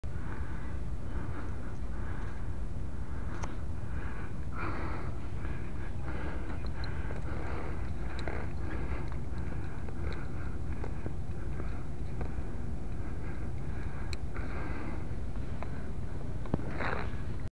Disconnecting a Sister Clip (Brummel Hook) from a Sail Cover
Sister Clips aka Brummel Hooks make it easy to disconnect lines from sail covers like this Sailrite Sail Pack.
Sister Clips